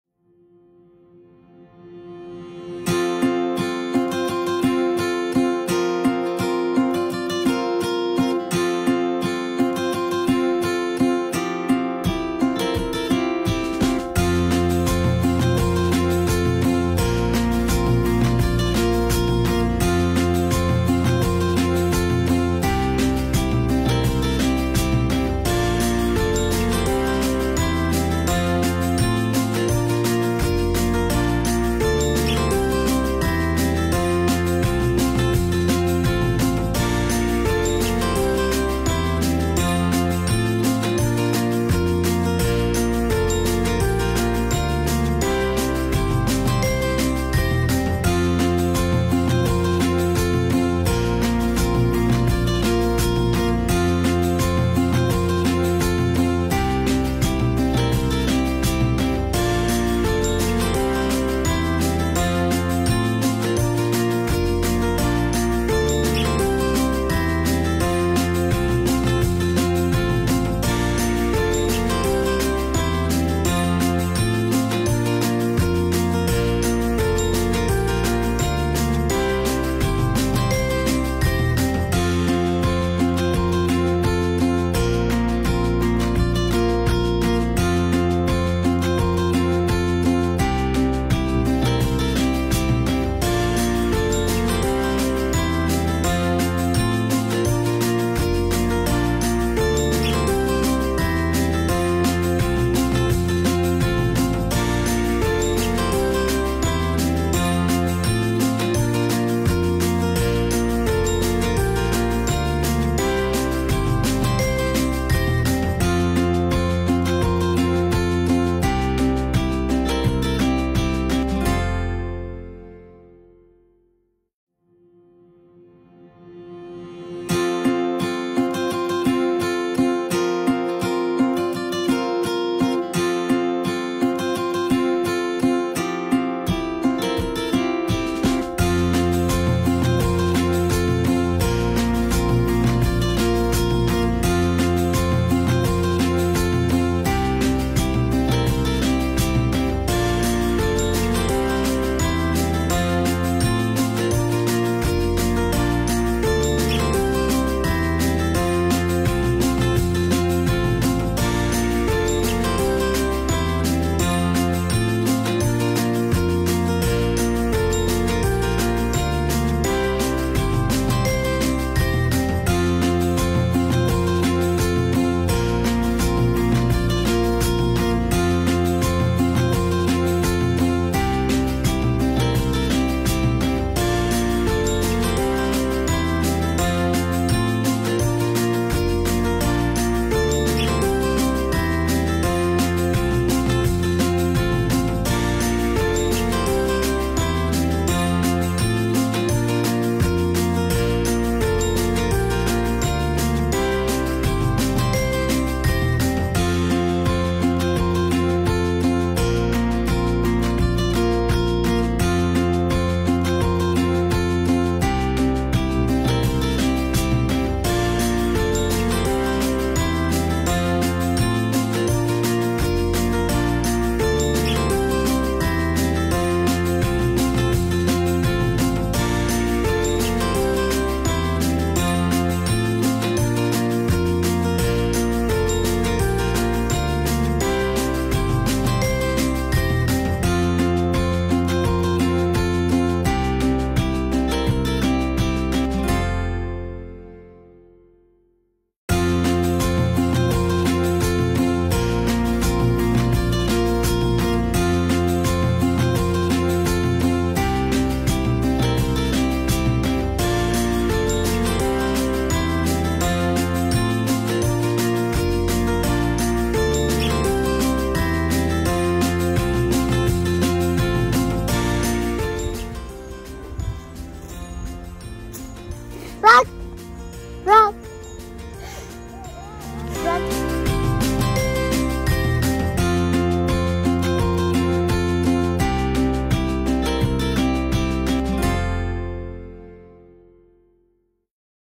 Karzakkan tree corridor
Away from the din of the city, a serene location with its captivating greenery, its location near the sea adds to the natural views. It is home to some beautiful birds that you would usually not see on the city side. A perfect weekend getaway for nature lovers. Don’t forget to watch out for horses enjoying their grassy meal.